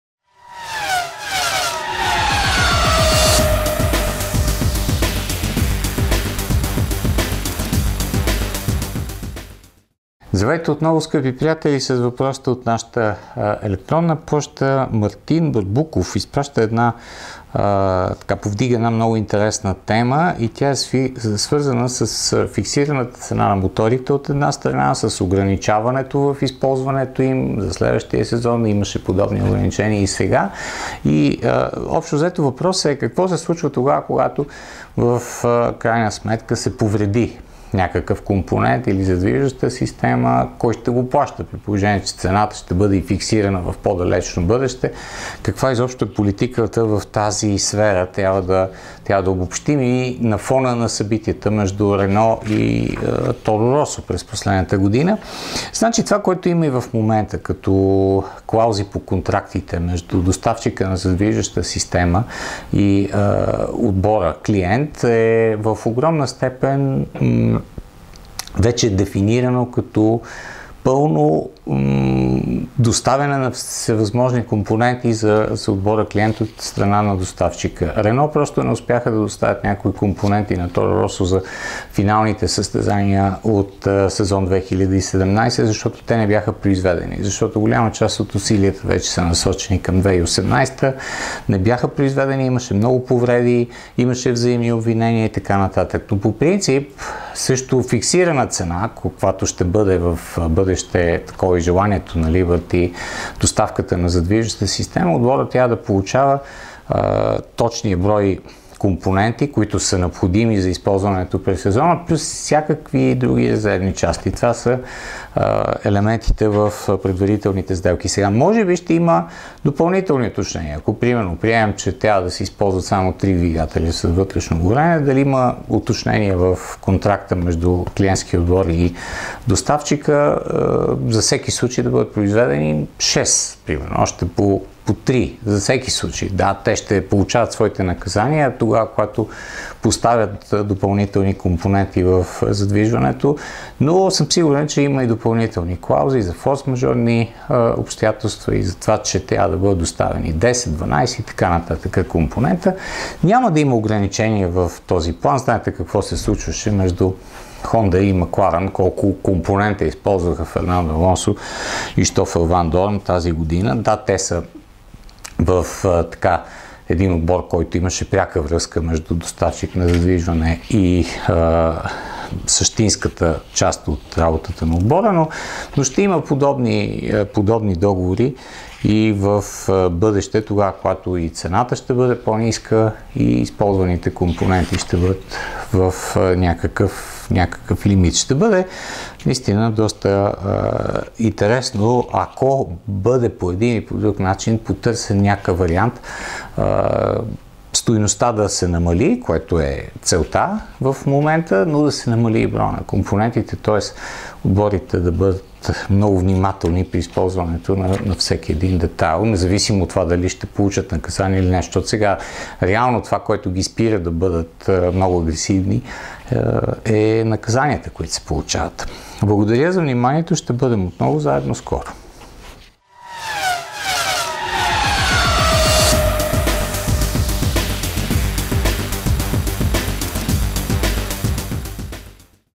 Двигателите, повредите и резервните части за тях
След като цената на моторите за един сезон е фиксирана, това означава ли, че при повреда допълнителните компоненти не се заплащат отделно? Т.е. Всеки клиент си купува на Х млн 4 задвижващи системи и каквото се повреди и не изкара нужните състезания се подменя безплатно?

Въпрос на Мартин Барбуков